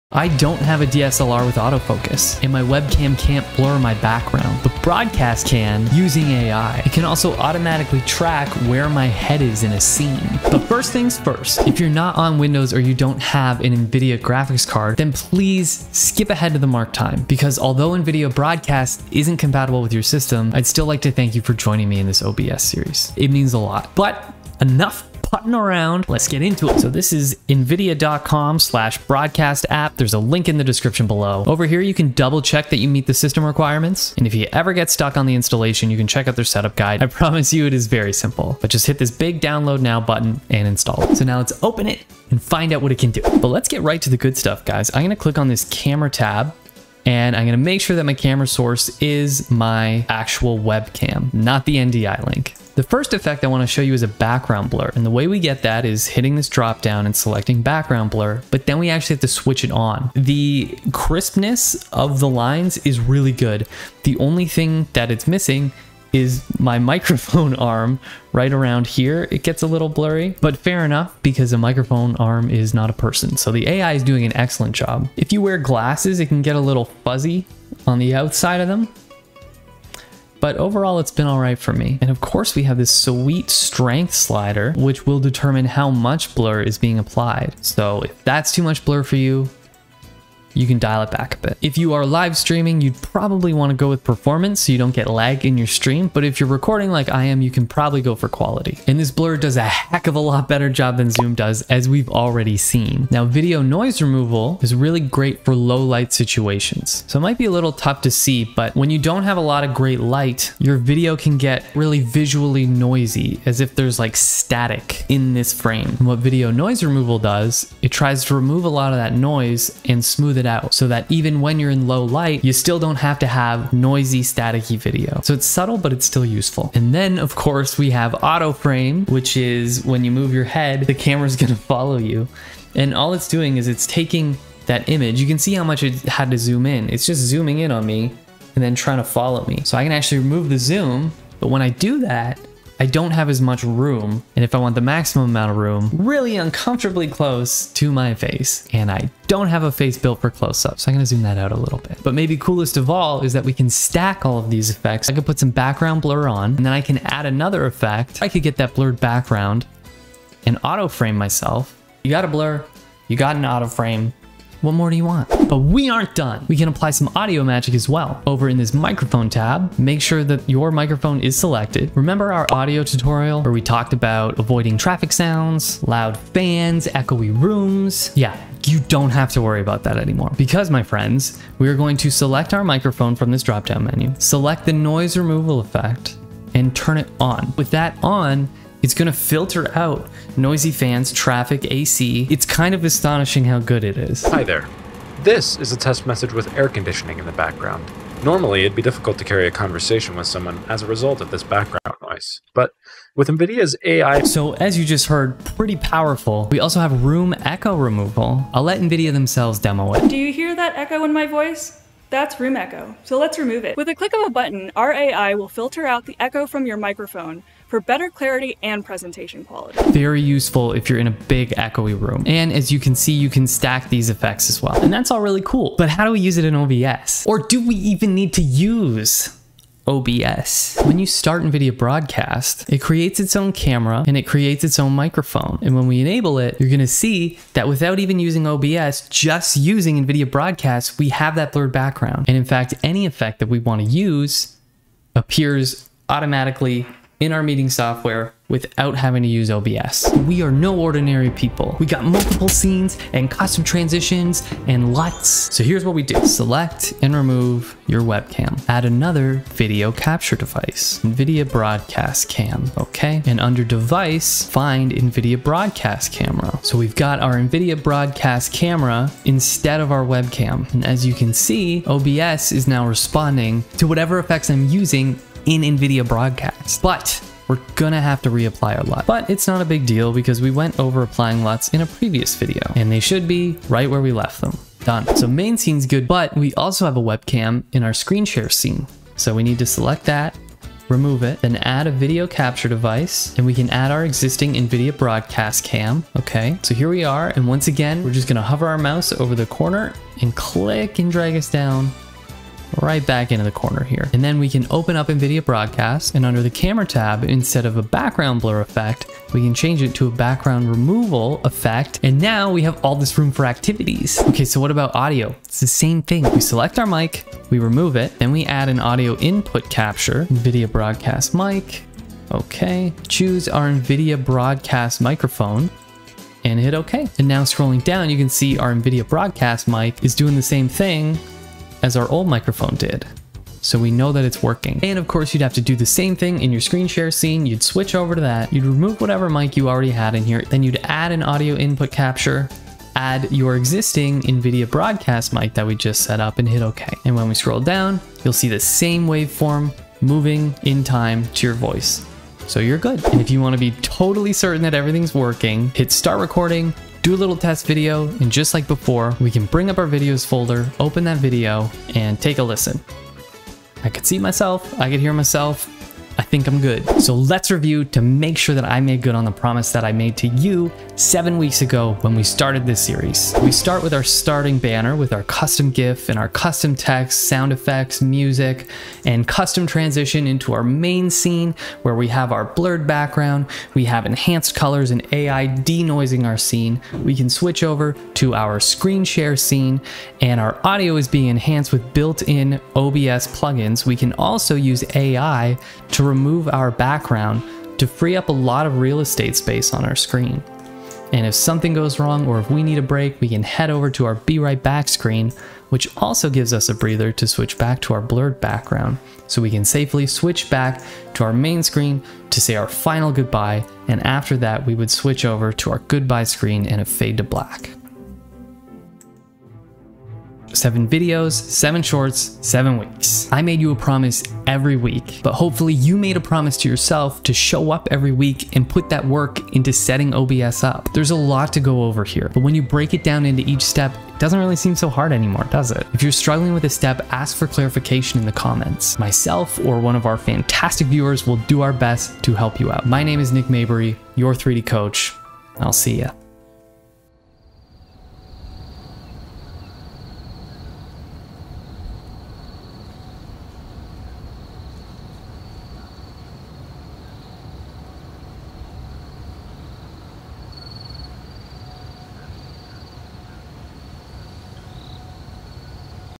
NVIDIA Broadcast Tutorial (2021)
Insane AI Effects in OBS using NVIDIA Broadcast 🤯

This NVIDIA Broadcast tutorial  for 2021 will teach you how to use NVIDIA Broadcast with OBS Studio so you can: Blur your background, green screen OBS without a green screen, camera and facial tracking, remove echo from your microphone, remove noise from your audio, and more.

Seriously, this is POWERFUL!

0:00 Demo
0:25 Downloading Broadcast
0:42 Applying effects
3:50 You don't need OBS
4:15 Broadcast video in OBS
5:13 Broadcast audio in OBS
6:06 What it all looks like
7:00 Thank you!

About this channel:
3D Coach is your community for digital creative hobbyists.